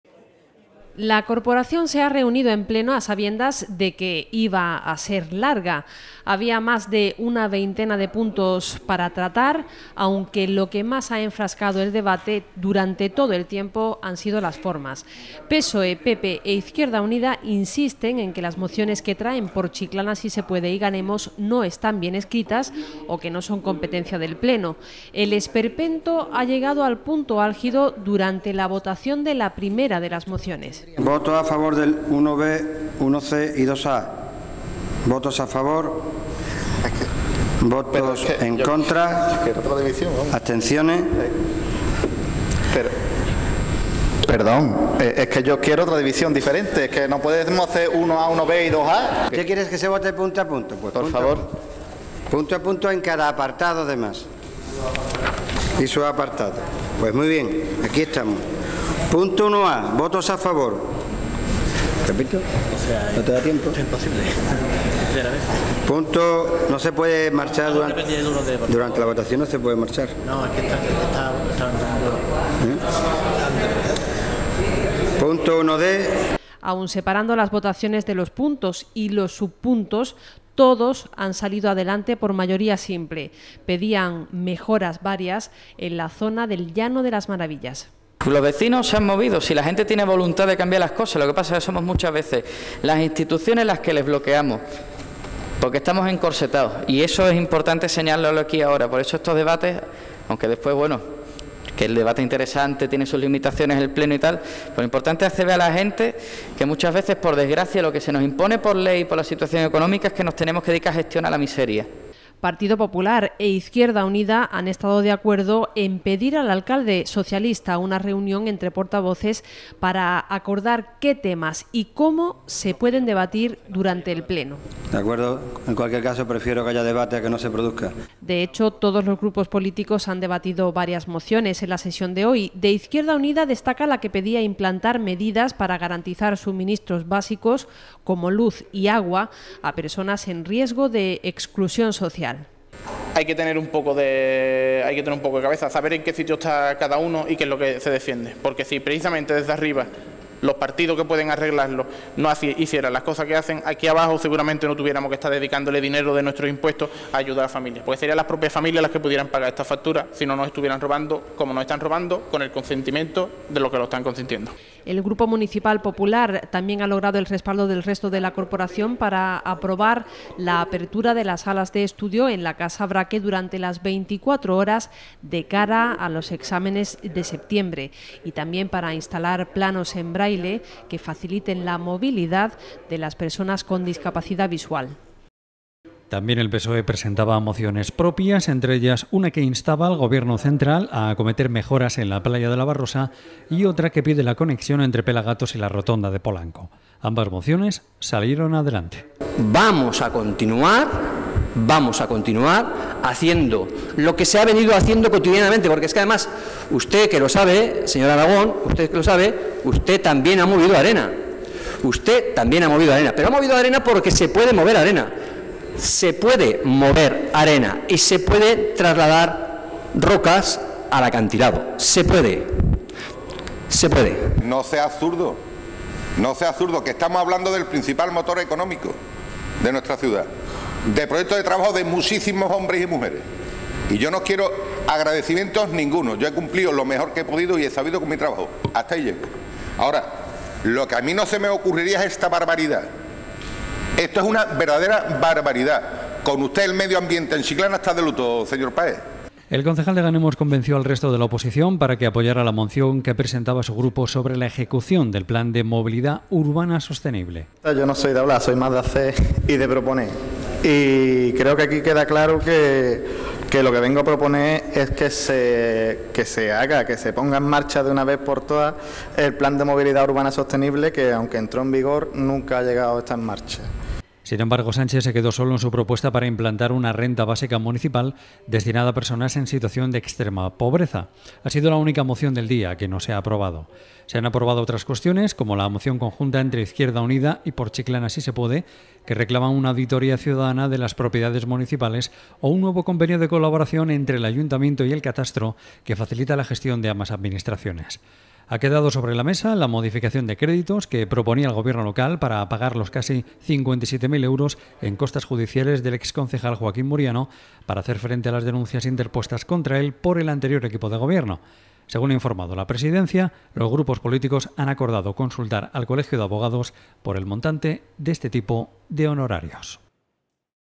PLENO AGOSTO
Este jueves, la atención política local se ha centrado en el pleno que se ha celebrado en el Ayuntamiento. Ha sido una sesión larga, con muchas propuestas y puntos para debatir, pero que la corporación ha desarrollado en un tono más o menos cordial y amable. Prácticamente, se han aprobado todos los puntos que figuraban en el orden del día. Eso sí, se nota la llegada de los nuevos grupos políticos.